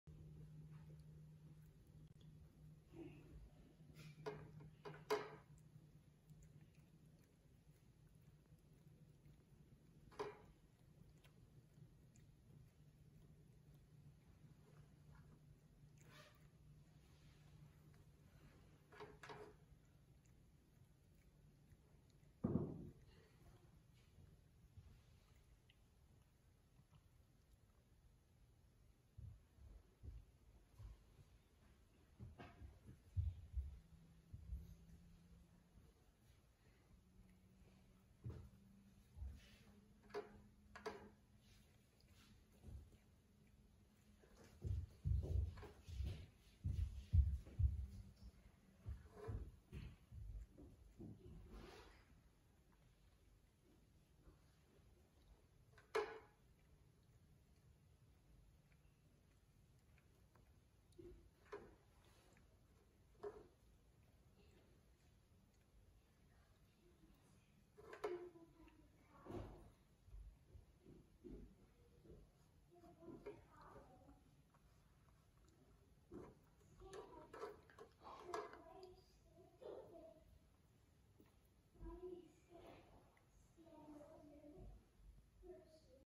Large Maine Coon Eats With His Paws
Here’s an adorable video of a Maine Coon cat eating with their paws.

Click on the link below, to learn everything there is to know about the Maine Coon Cat breed:

Appreciate Pippin's content?